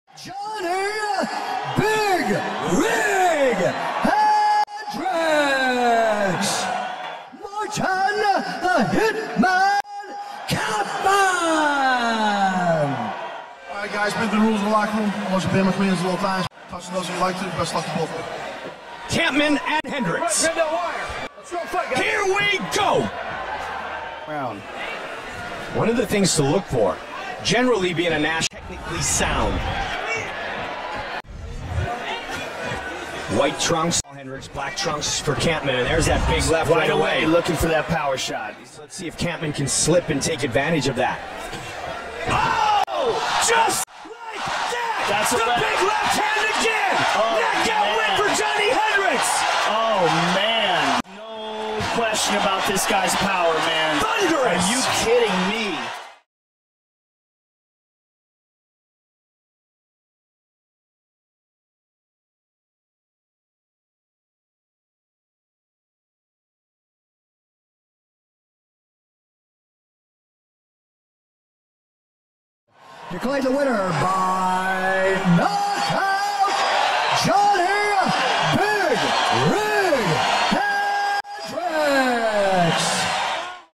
Johny Hendricks vs Martin Kampmann Highlights (Thunderous KNOCKOUT)
UFC 154: St-Pierre vs. Condit was a mixed martial arts pay-per-view event held by the Ultimate Fighting Championship on November 17, 2012, at the Bell Centre in Montreal, Quebec, Canada.

Johny Hendricks faced Martin Kampmann on November 17, 2012, at UFC 154. Hendricks won via KO in the first round, with the fight only lasting 46 seconds. He earned Knockout of the Night honors for his performance.